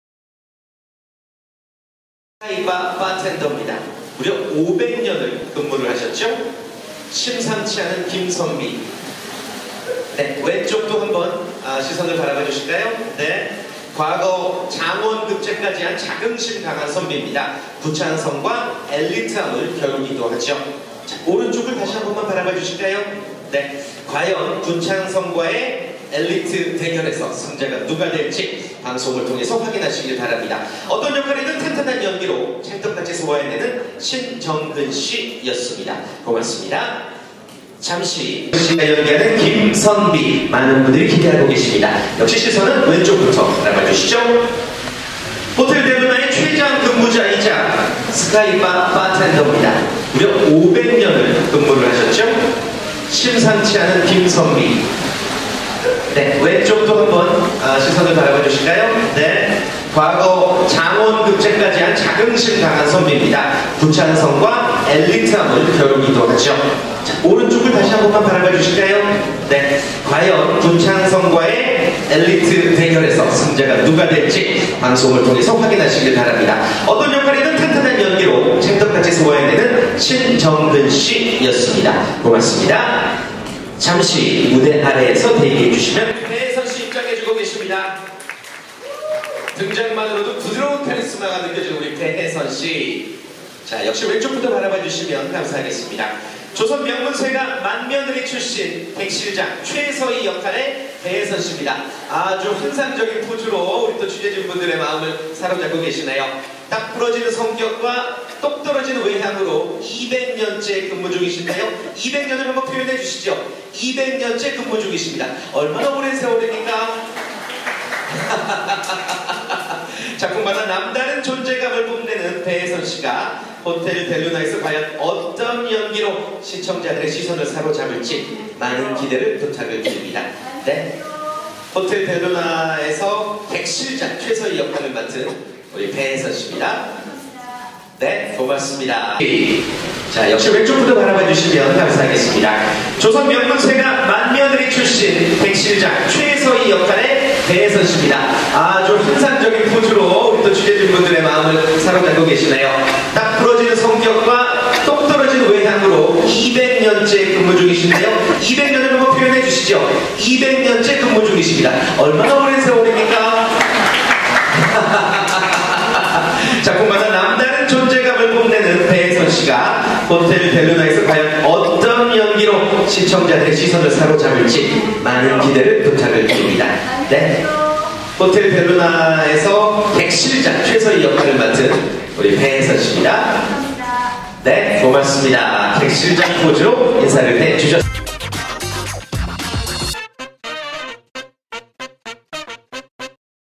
190708 귀신이 호텔직원? 배해선X신정근, 포토타임 (tvN '호텔 델루나' 제작발표)
8일 오후 서울시 강남구 논현동 임피리얼 팰리스 서울에서 tvN 새 토일 드라마 ‘호텔 델루나’ 제작발표회가 열려 배해선, 신정근이 참석해 포토타임을 갖고 있다.

tvN '호텔 델루나'는 엘리트 호텔리어가 운명적인 사건으로 호텔 델루나의 지배인을 맡게 되면서 달처럼 고고하고 아름답지만 괴팍한 사장과 함께 델루나를 운영하며 생기는 특별한 이야기를 그린 판타지 호로맨스 드라마로 오는 13일 오후 9시 첫 방송한다.